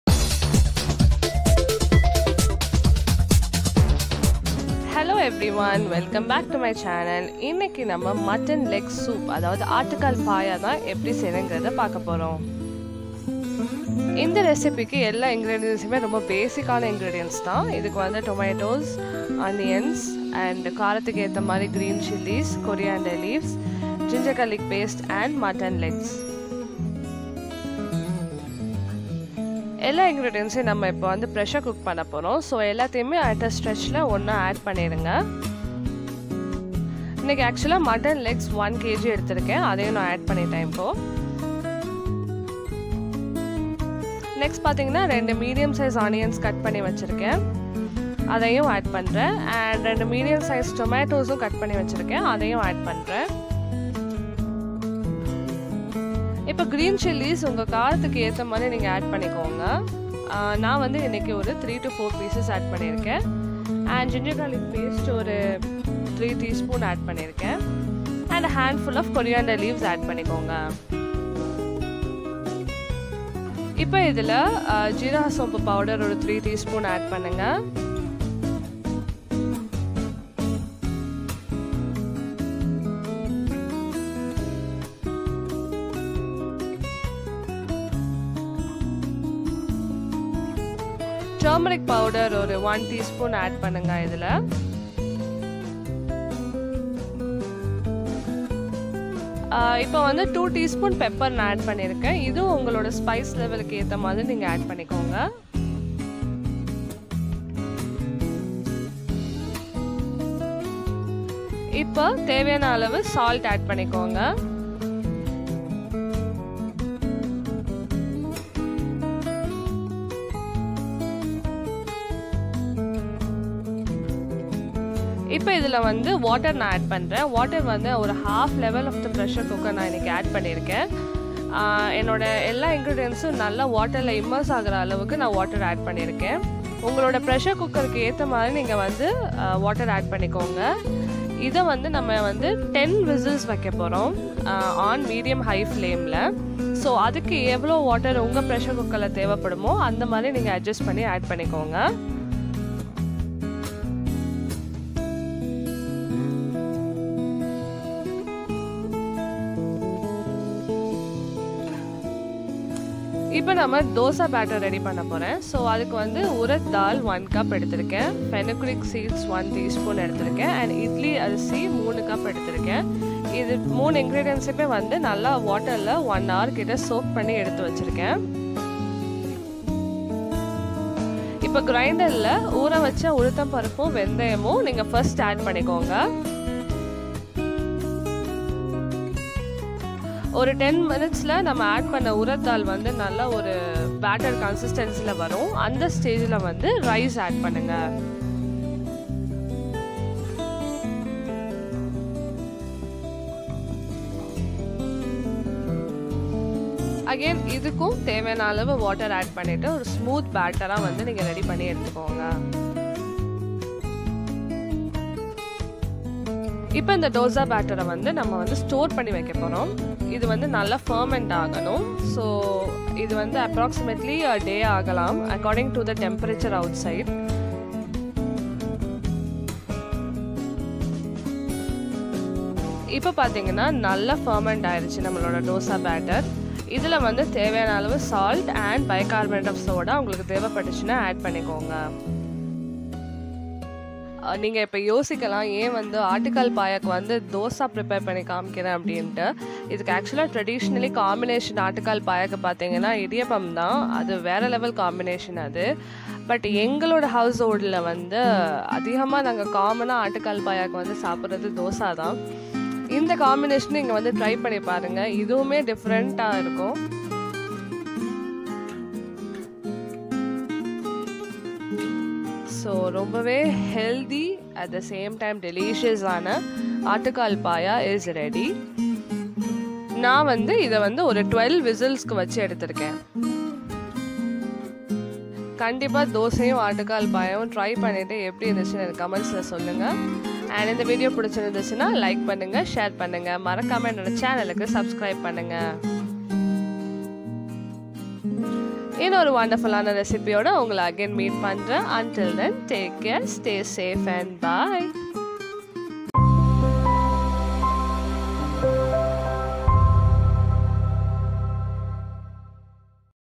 MUTTON LEG SOUP | Aattukaal Paaya | Indian Cuisine | Zafrin Banu
Mutton leg soup - a traditional dish on the line up. This a healthy soup which can be had with Idiyappam and dosa.

Ingredients
Mutton (leg) - 1kg
Green chilli - 3 to 4 pc
Coriander leaves
Fennel/cumin powder - 3 tsp
Pepper - 2 tsp
Salt

For dosa batter
Urad dal - 1 cup
Idly Rice - 3 cup
Fenugreek seeds - 1 tsp
Bicarbonate of soda & salt

Musician:  Ilya Truhanov
Music Bossanova
Musician: Ilya Truhanov